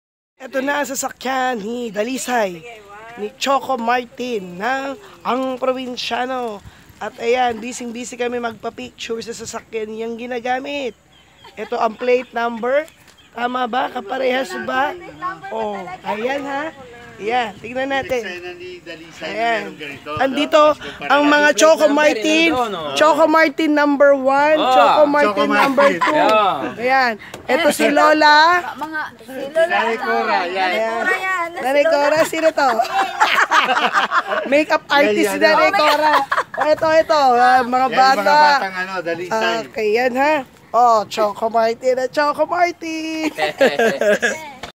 Ang Probinsyano: sa likod ng Camera
Mga kadouble ni coco martin: si choco at cocoa martin with RAMBO